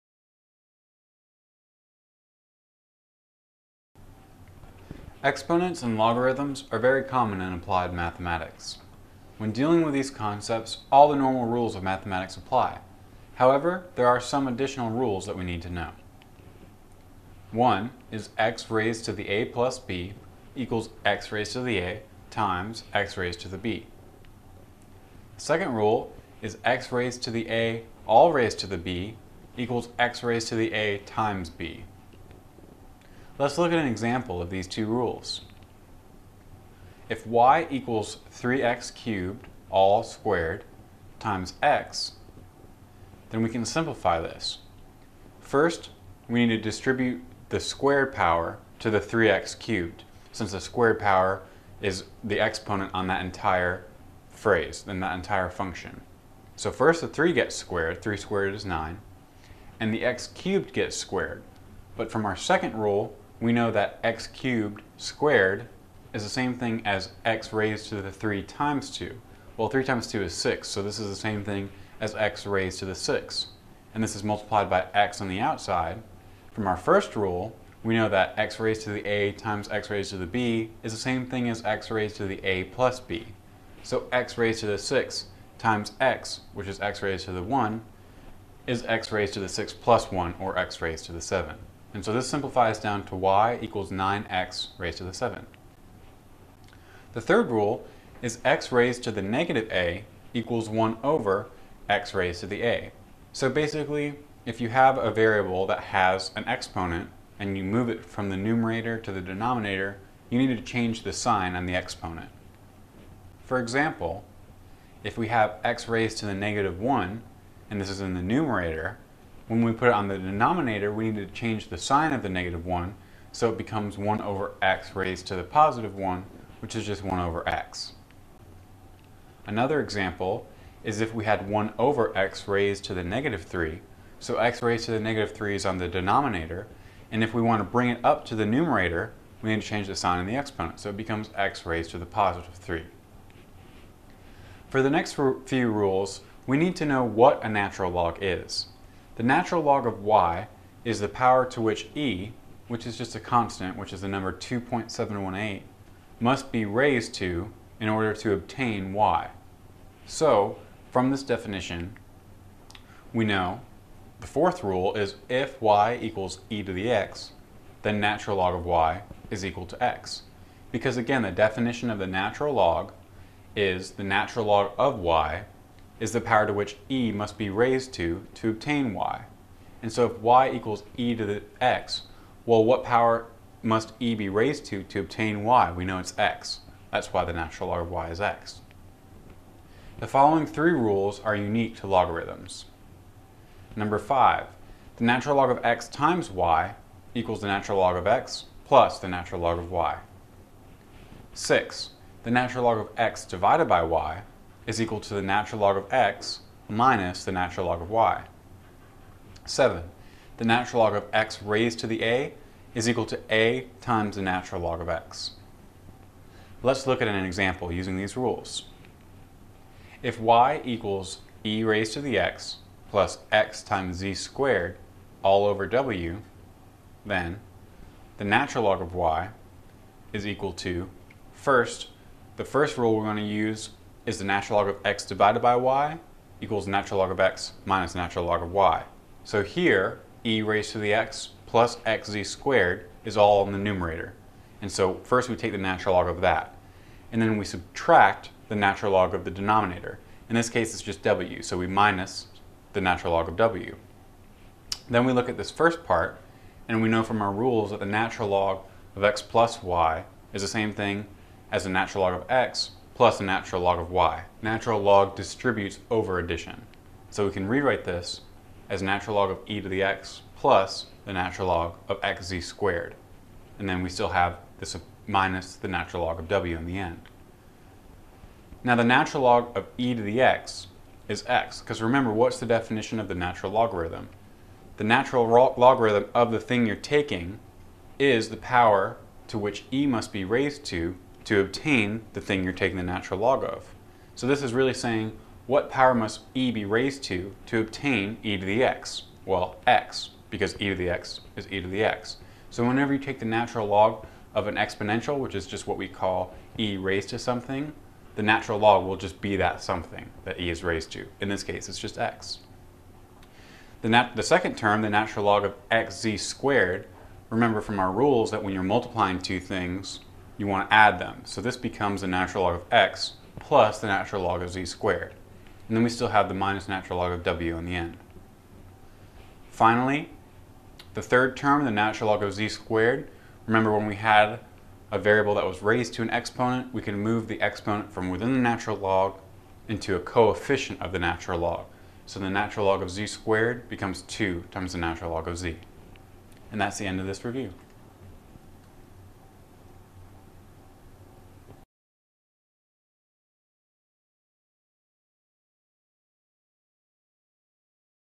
Math Review Part 5 Exponents and Logarithms
This is Part 5 of our Math Review. It deals with the rules of exponents and logarithms.